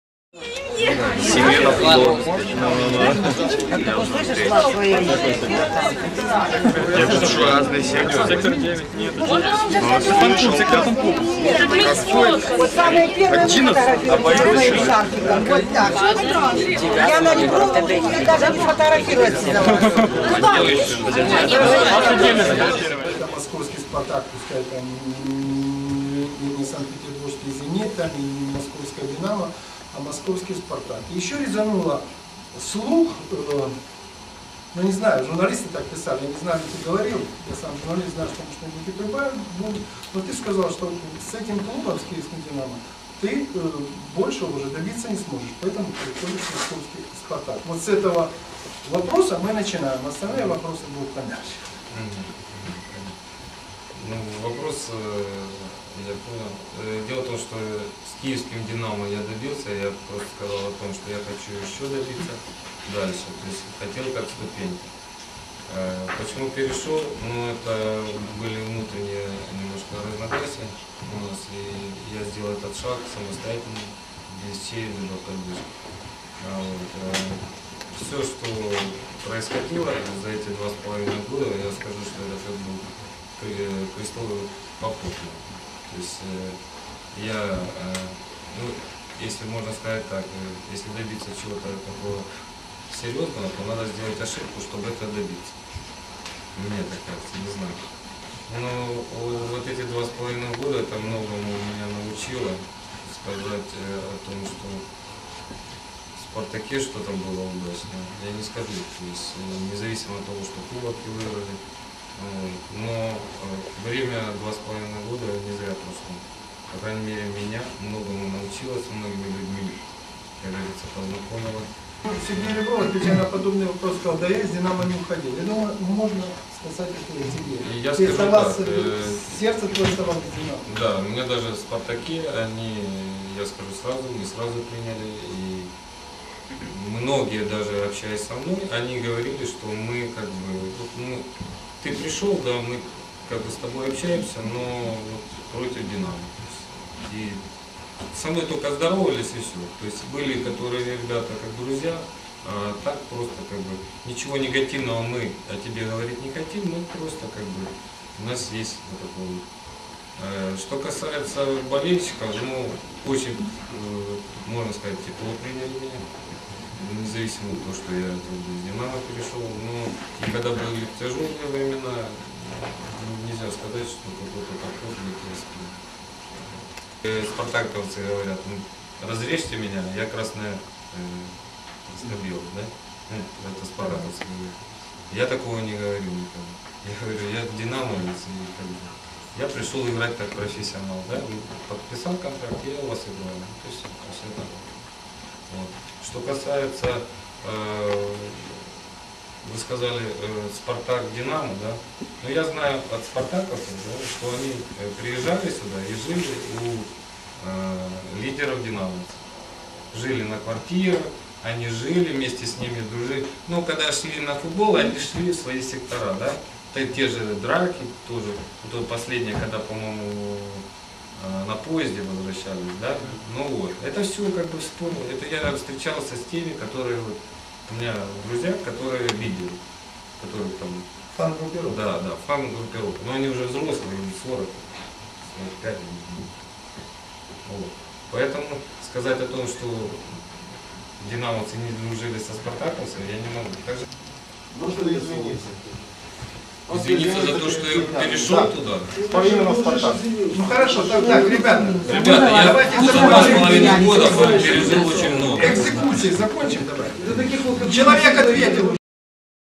Ващук о переходе в Спартак_2005
Владислав Ващук про перехід в московський спартак.